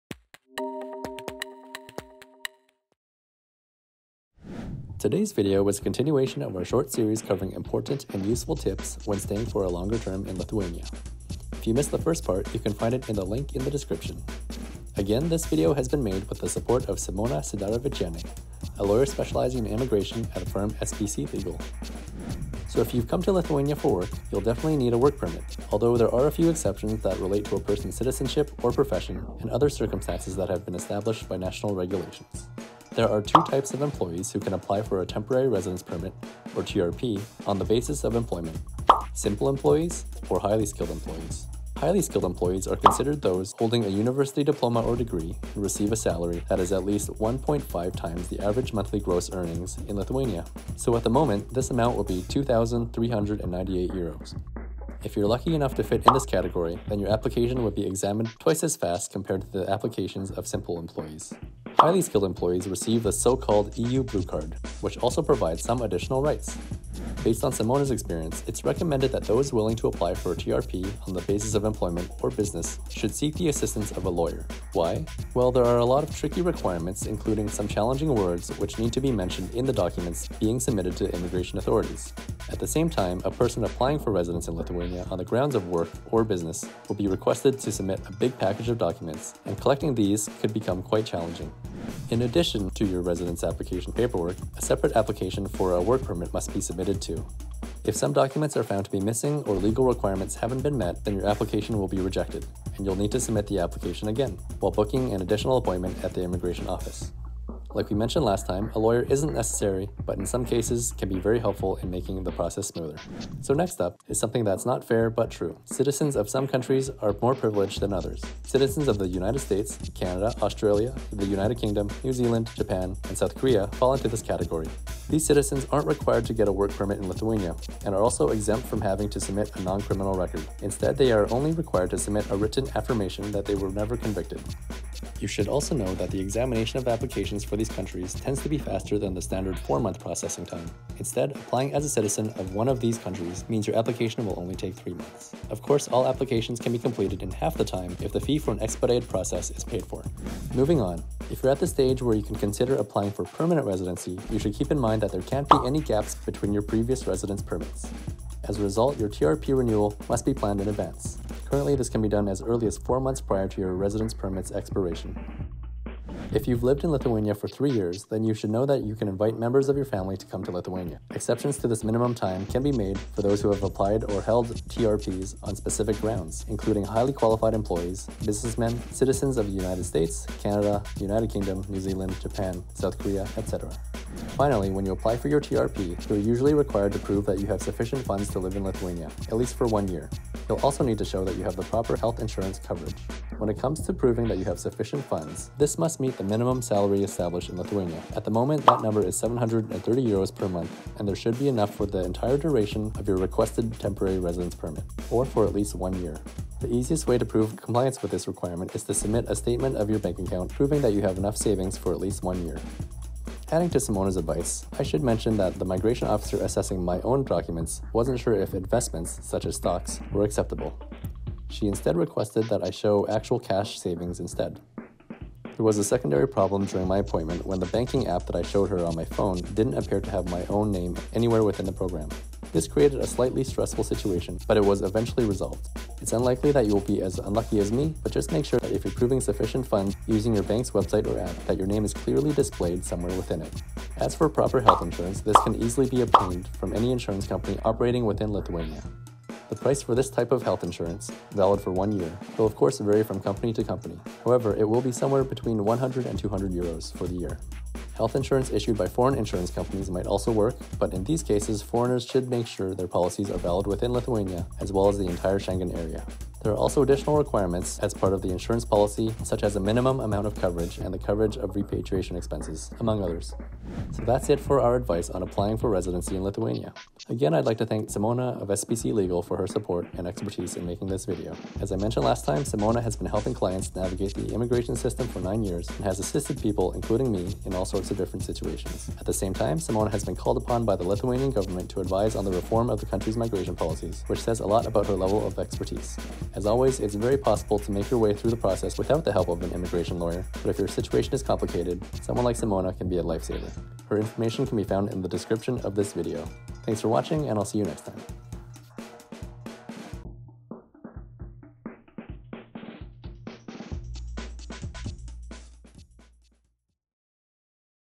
TRPs, Work Permits, & Special Exceptions: More Migration Tips for Lithuania
Today’s video is a continuation of our short series covering important and useful tips when staying for a longer term in Lithuania. If you missed the first part - you can find it

Again, this video has been made with the support of Simona Sidaravičienė, a lawyer specializing in immigration at the firm SPC Legal.

Follow this YouTube channel through Instagram to find out what's new and what future videos are planned!